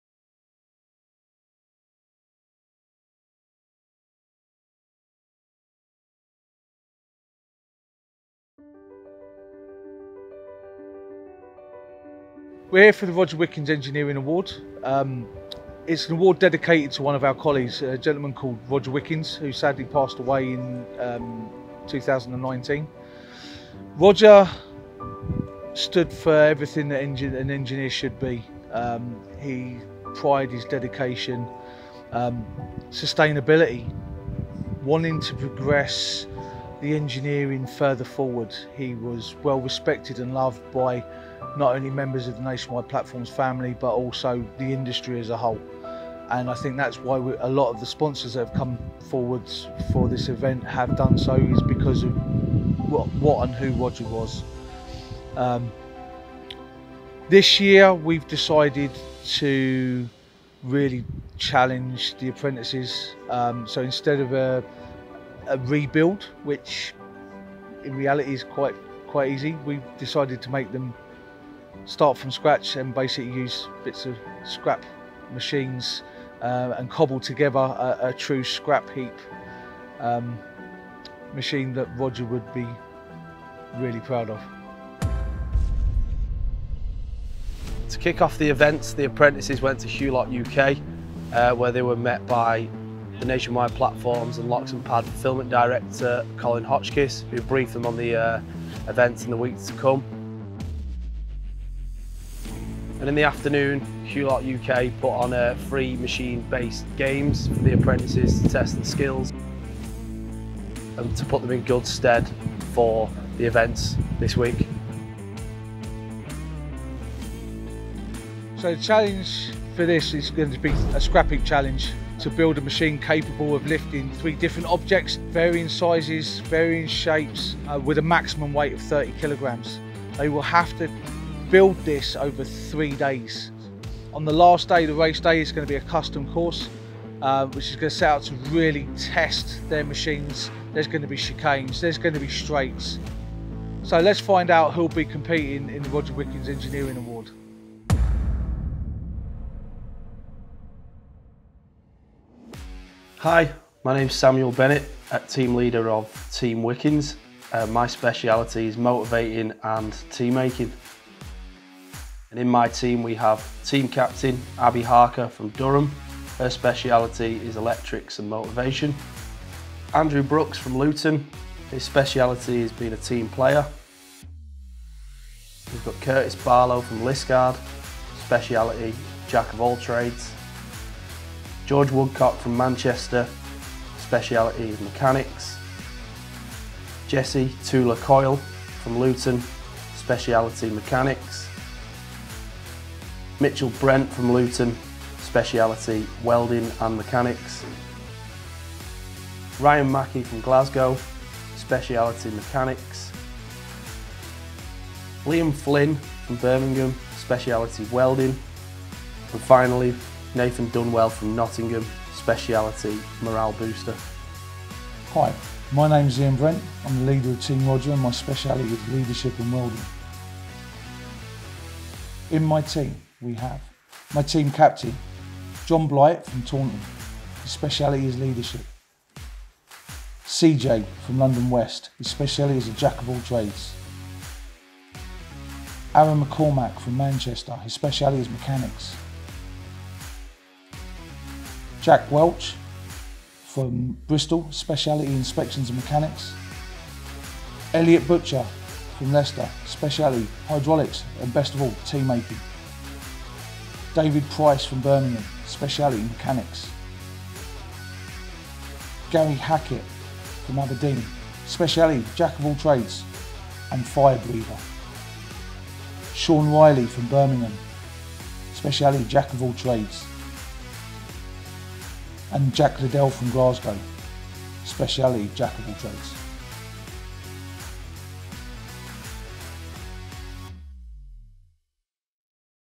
Nationwide Platforms: Roger Wickens Engineering Award 2021 - Intro
Nationwide Platforms apprentices gathered this year to build their own specialist machines from spare parts to compete for the Roger Wickens Engineering Award 2021!